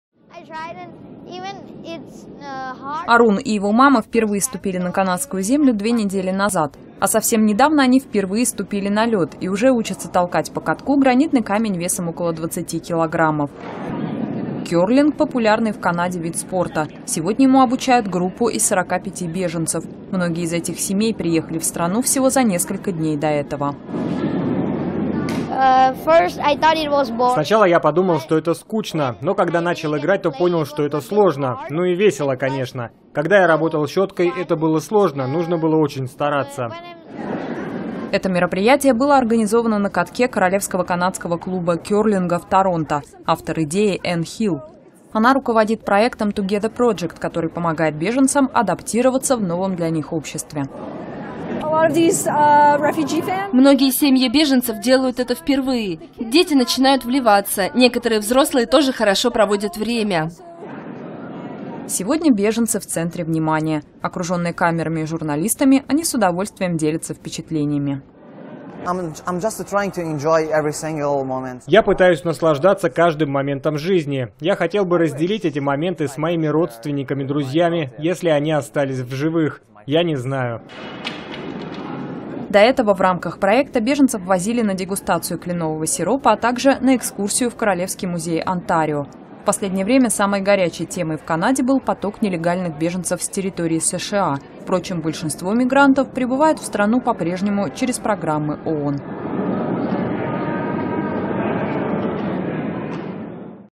Канада: беженцы адаптируются при помощи кёрлинга (новости)
Канада: беженцы адаптируются при помощи кёрлинга. Арун и его мама впервые ступили на канадскую землю две недели назад. А совсем недавно они впервые ступили на лёд и уже учатся толкать по катку гранитный камень весом около 20 килограммов.

Кёрлинг – популярный в Канаде вид спорта. Сегодня ему обучают группу из 45 беженцев. Многие из этих семей приехали в страну всего за несколько дней до этого.

[Арун Даниэль, беженец из Шри-Ланки]:
«Сначала я подумал, что это скучно. Но когда начал играть, то понял, что это сложно. Ну и весело, конечно. Когда я работал щёткой, это было сложно, нужно было очень стараться».

Это мероприятие было организовано на катке Королевского канадского клуба кёрлинга в Торонто. Автор идеи – Энн Хилл. Она руководит проектом Together Project, который помогает беженцам адаптироваться в новом для них обществе.

[Энн Хилл, руководитель проекта Together Project]:
«Многие семьи беженцев делают это впервые. Дети начинают вливаться, некоторые взрослые тоже хорошо проводят время».

Сегодня беженцы - в центре внимания. Окружённые камерами и журналистами, они с удовольствием делятся впечатлениями.

[Карам Джамало, беженец из Сирии]:
«Я пытаюсь наслаждаться каждым моментом жизни. Я хотел бы разделить эти моменты с моими родственниками, друзьями, если они остались в живых... я не знаю».

До этого в рамках проекта беженцев возили на дегустацию кленового сиропа, а также на экскурсию в Королевский музей Онтарио.

В последнее время самой горячей темой в Канаде был поток нелегальных беженцев с территории США. Впрочем, большинство мигрантов прибывают в страну по-прежнему через программы ООН.